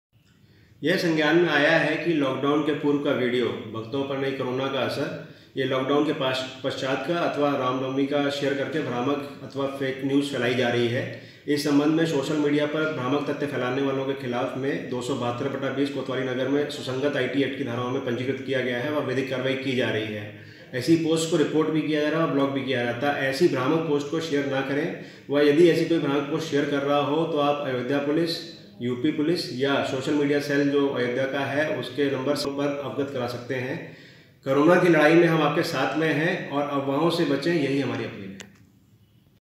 अयोध्या में फेक वीडियो वायरल मामले पर एसएसपी हुए शख्त
*अयोध्या के वीडियो वायरल होने के मामले में एसएसपी का बयान*

लॉक डाउन के पहले का वीडियो हो रहा वायरल- एसएसपी अयोध्या

फेक वीडियो वायरल करने वालों पर कानूनी कार्रवाई कर रहे हैं- एसएसपी अयोध्या

फेक वीडियो फैलाने वालों पर कार्यवाही हो रही है- एसएसपी अयोध्या

कोई फेक वीडियो आपको पता चलता है तो अयोध्या पुलिस और यूपी पुलिस को सूचित करें- एसएसपी अयोध्या

कोरोना की लड़ाई में हम आपके साथ हैं-एसएसपी अयोध्या

18 मार्च और पहले के वीडियो वायरल किया जा रहा- एसएसपी अयोध्या